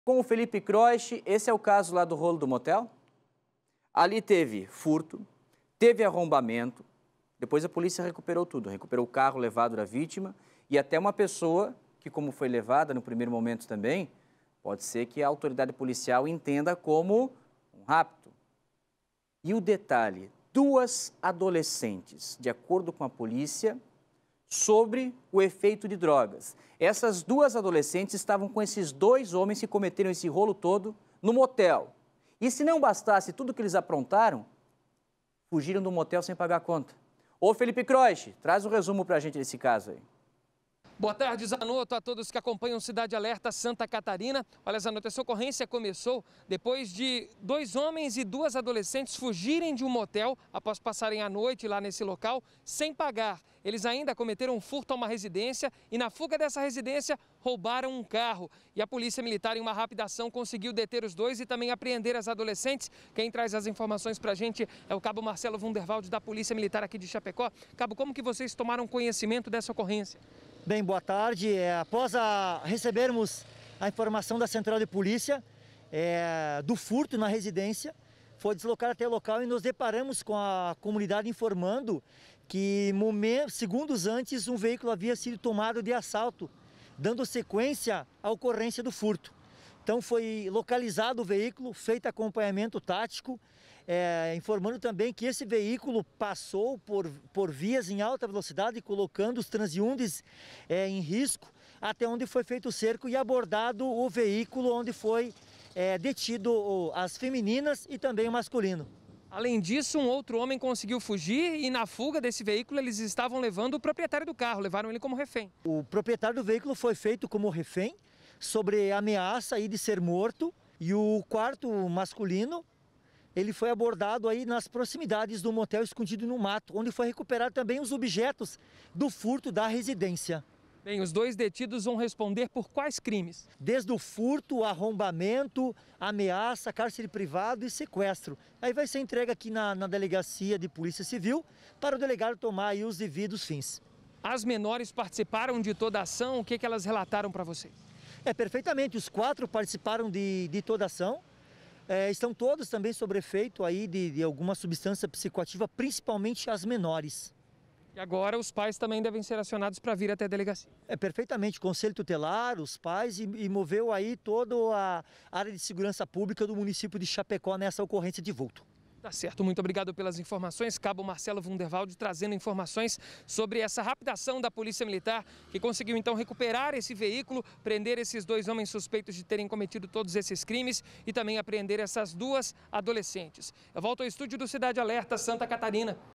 Polícia Militar prende suspeitos de fugirem de motel sem pagar a conta
Em Chapecó, no Oeste do estado, a Polícia Militar prendeu suspeitos de fugirem de um motel sem pagar a conta, furtar uma casa e roubar um veículo.

Confira mais detalhes na reportagem.